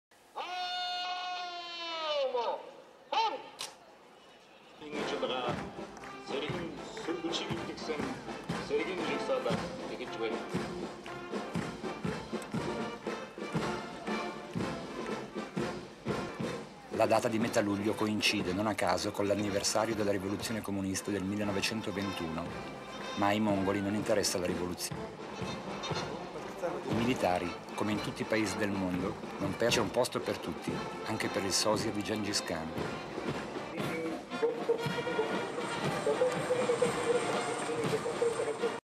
Highlights of Mongolian National Day Parade 1996
Some very rare footage detailing highlights from the parade on Ulaanbaatar's National Stadium commemorating the 75th anniversary of the Mongolian People's Revolution, 10 July 1996.